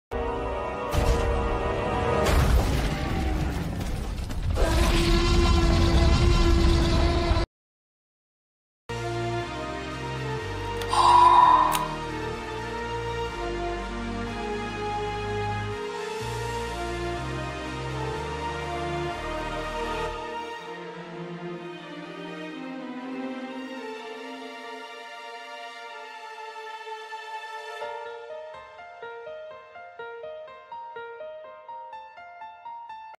Jurassic World Camp Cretaceous | Slash 'n Battle Scorpius Rex | Figure Movements | Mark7 Studios
On Friday will be released the review and the stop motion from this figure